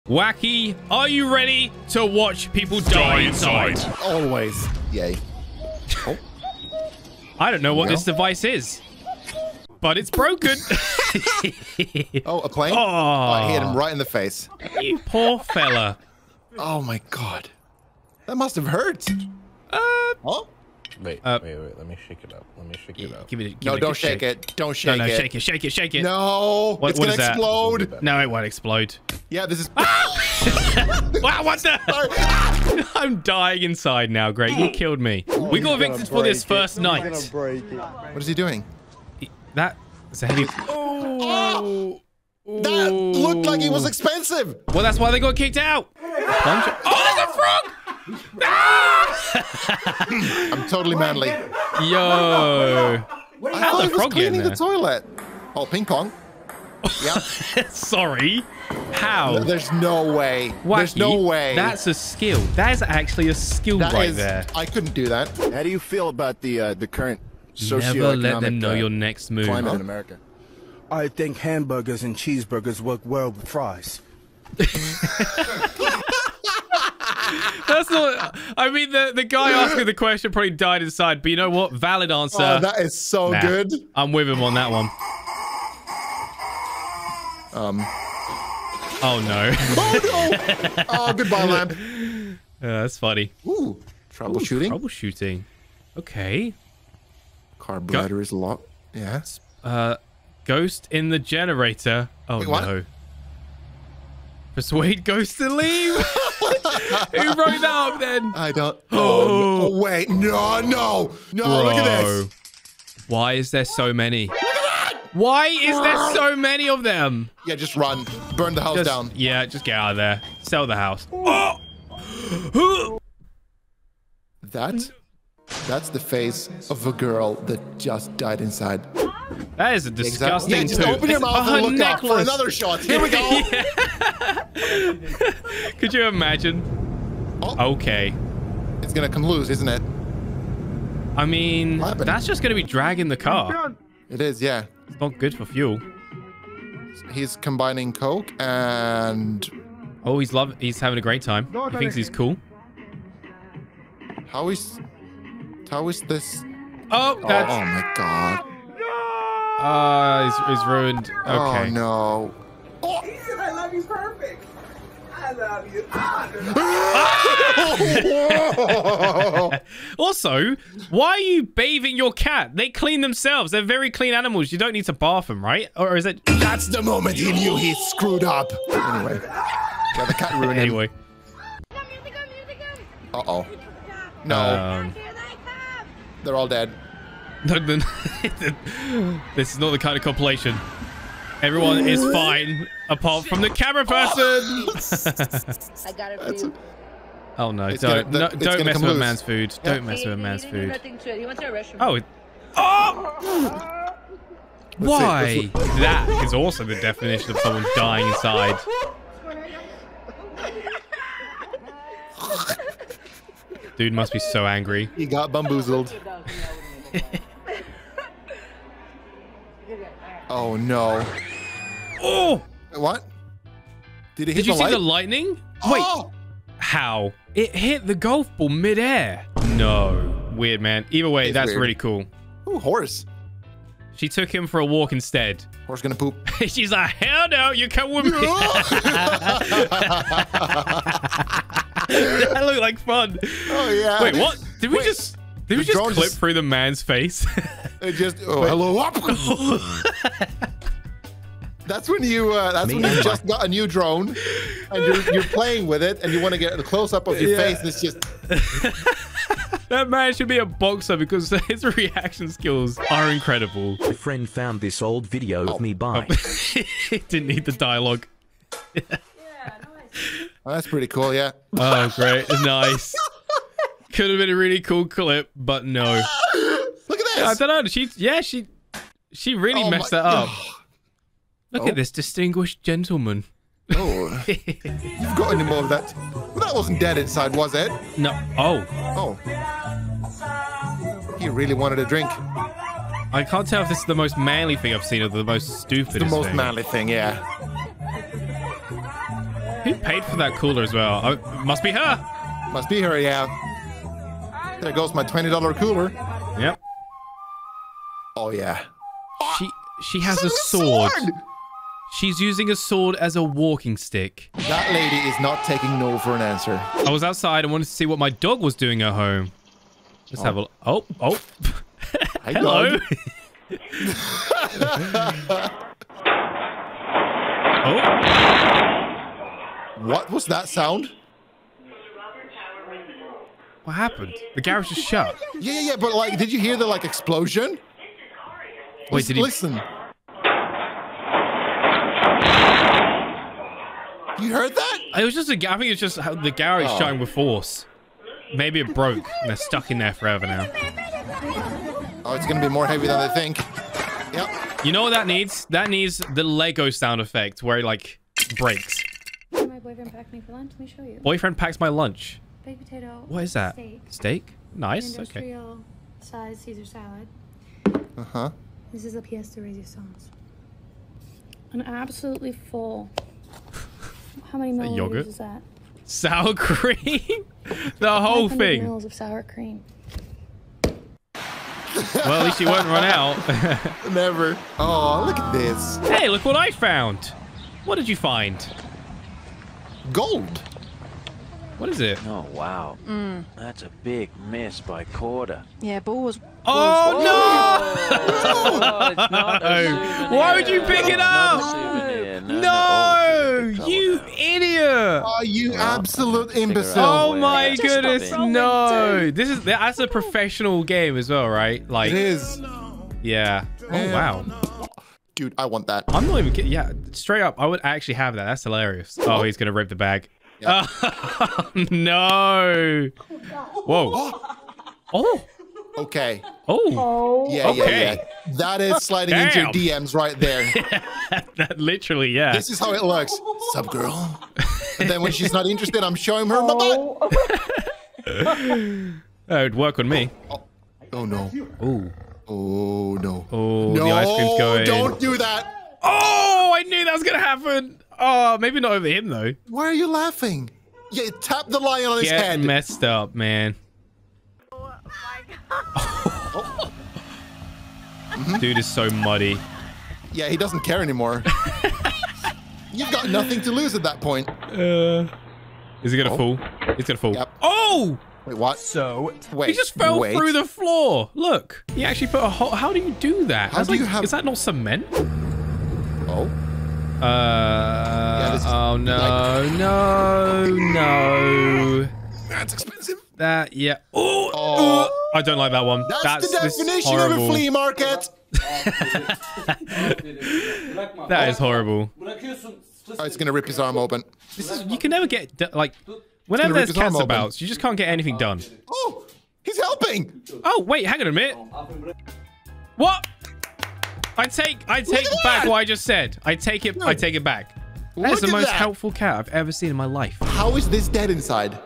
You Won't BELIEVE What This Lion Did Next (Watch People Die Inside)
Today Whacky and I are reacting to people dying inside. These memes and Tik Toks are full of fails. Let us know which one you think is the biggest fail! :)